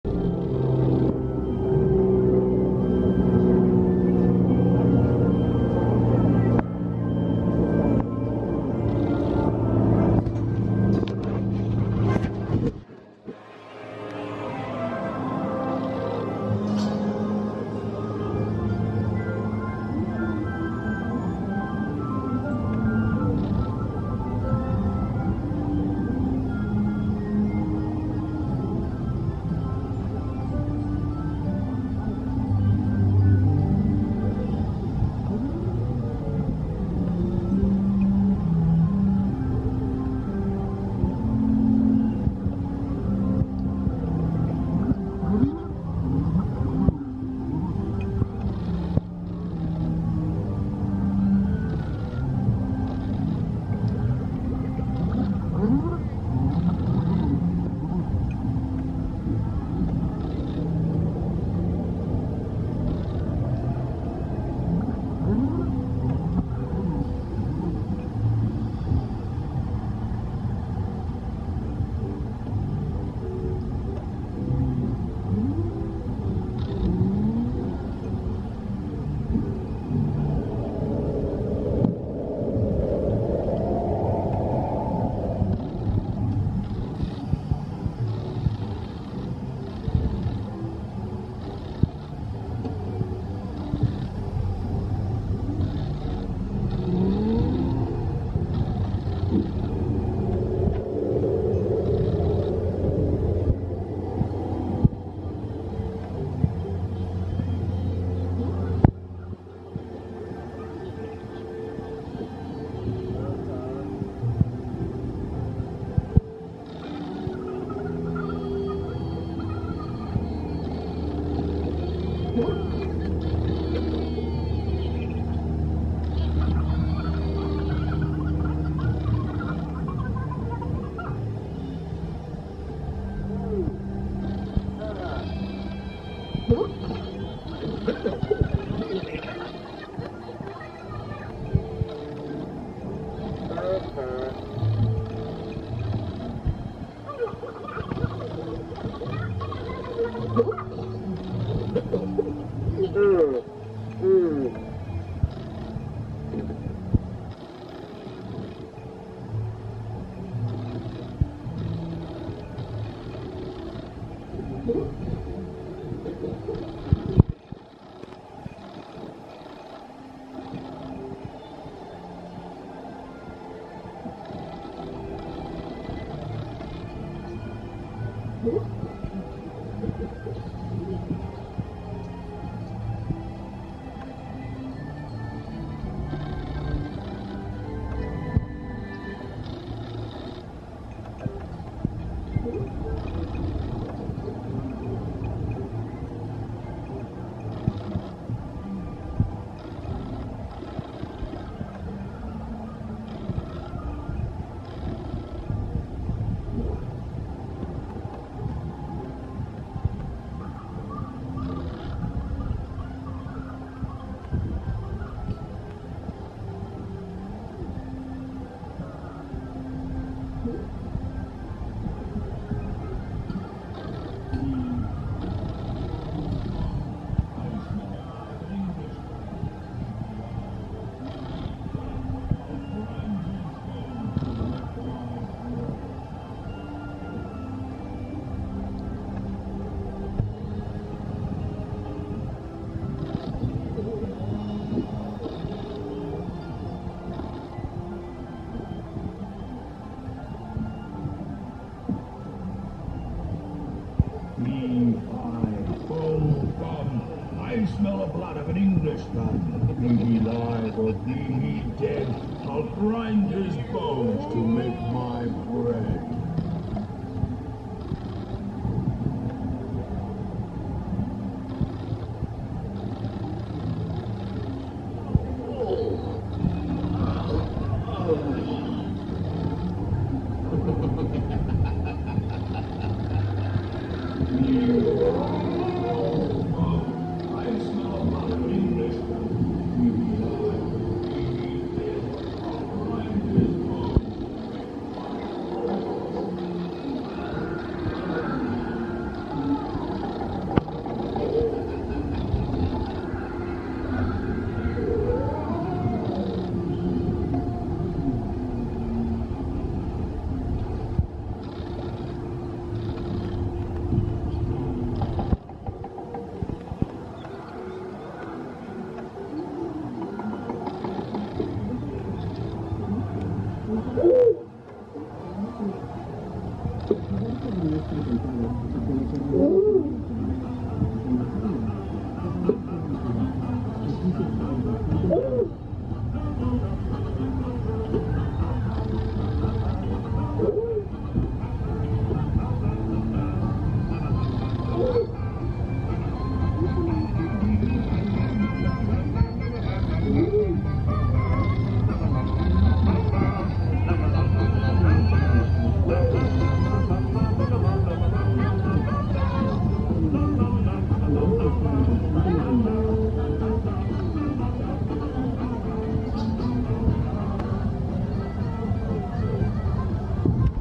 Fairy Tale Brook POV, Legoland California Boat Ride | Non-Copyright
Fairy Tale Brook is a slow-moving boat ride at Legoland California.  This ride takes you past several iconic fairy tales.